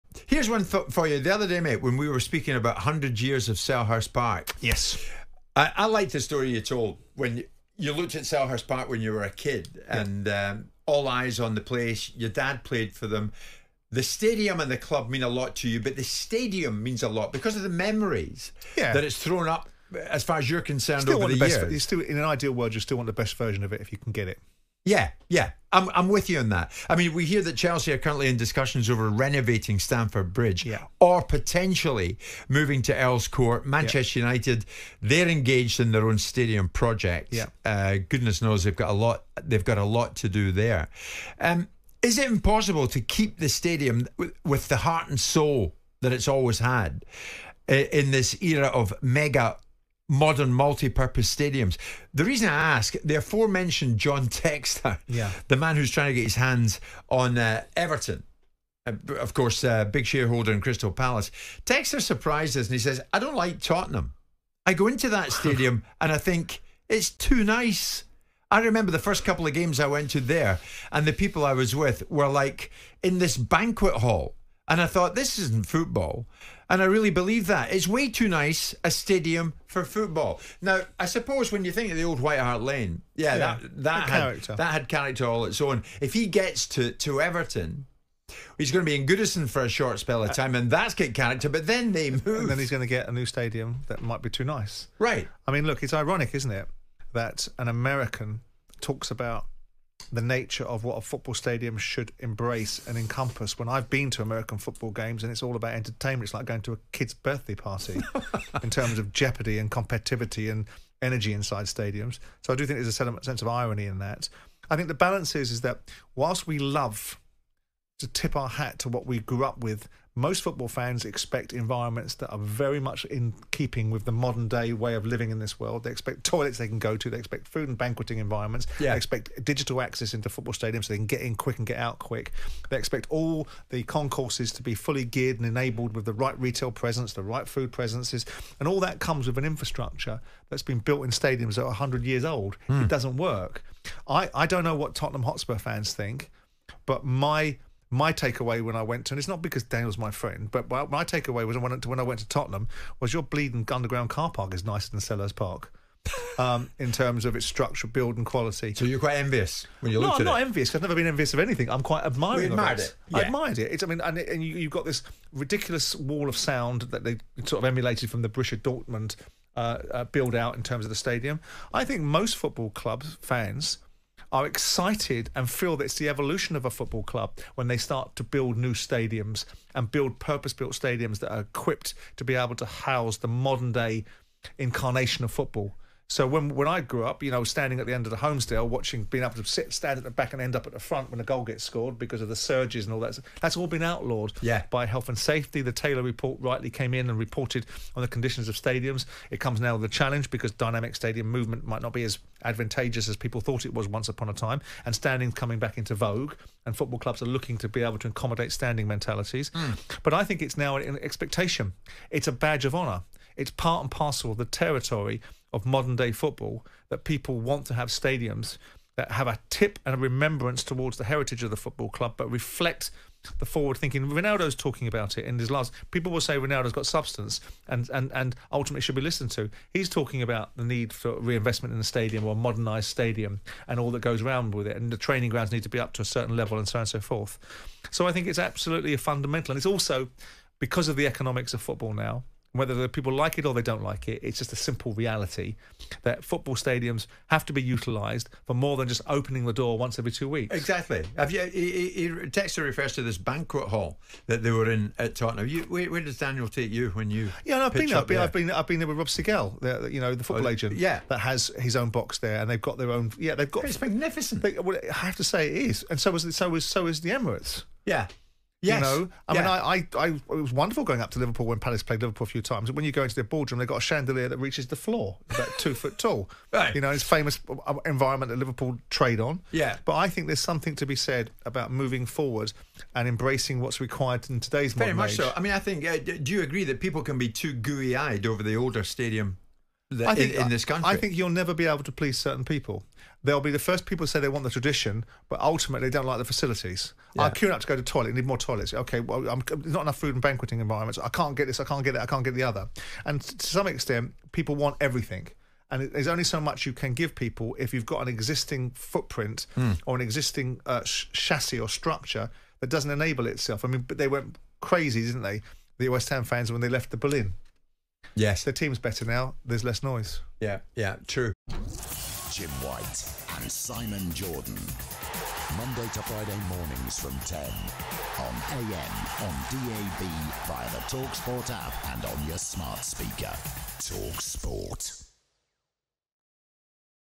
Simon Jordan INSISTS Tottenham Hotspur Stadium MUST Be Admired After Being Labelled As 'TOO NICE!' 👀
talkSPORT's Simon Jordan and Jim White discuss whether the Tottenham Hotspur Stadium is 'too nice' for football.